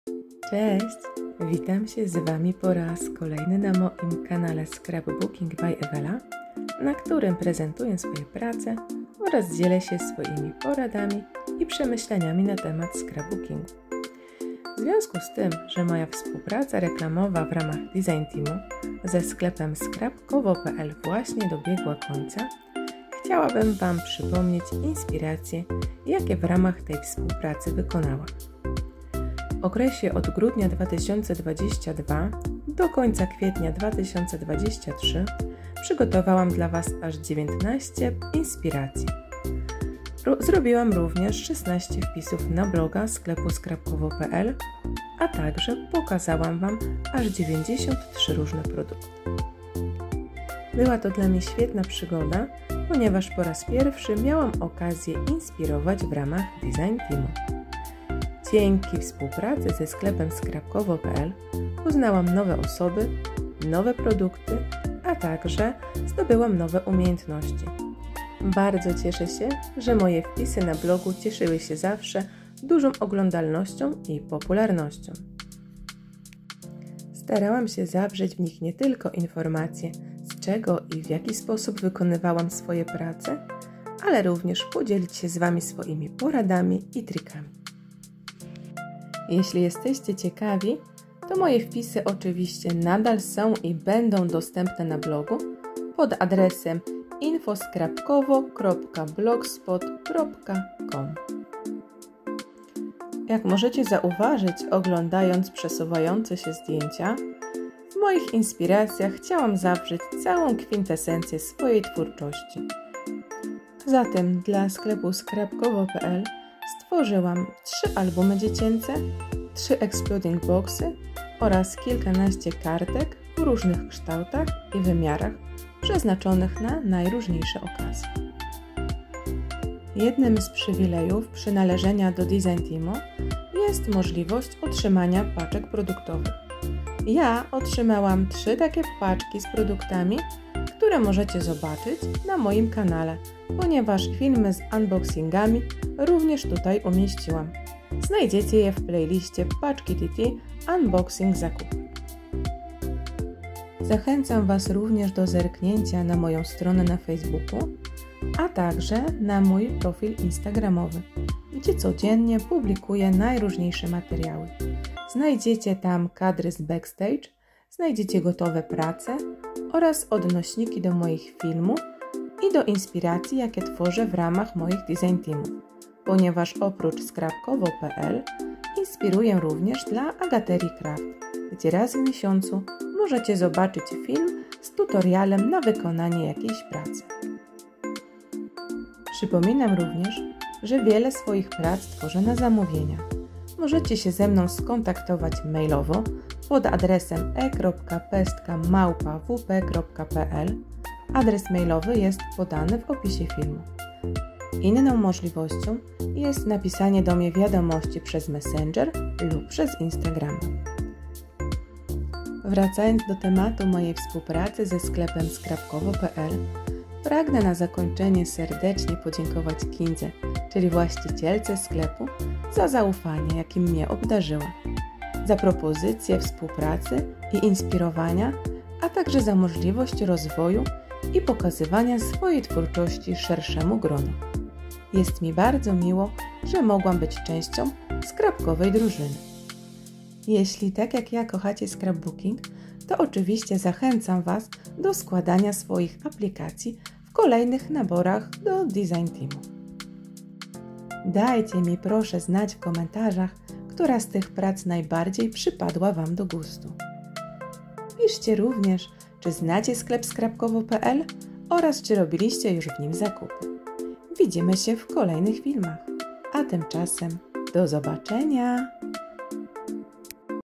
Podsumowanie współpracy ze sklepem Scrapkowo.pl w ramach design teamu
Cześć! Dzisiaj zapraszam Was serdecznie na prezentację wszystkich prac, jakie wykonałam w ramach DT sklepu Scrapkowo.pl.  Jest to podsumowanie pięciomiesięcznej owocnej współpracy,  która właśnie dobiega końca. Dziękuję za możliwość zaufanie i możliwość rozwoju 😀

/współpraca reklamowa/

oraz na instagramie @scrapbooking.by.evela

Chętnie wykonam kartki, albumy i inne prace na zamówienie!
/autopromocja/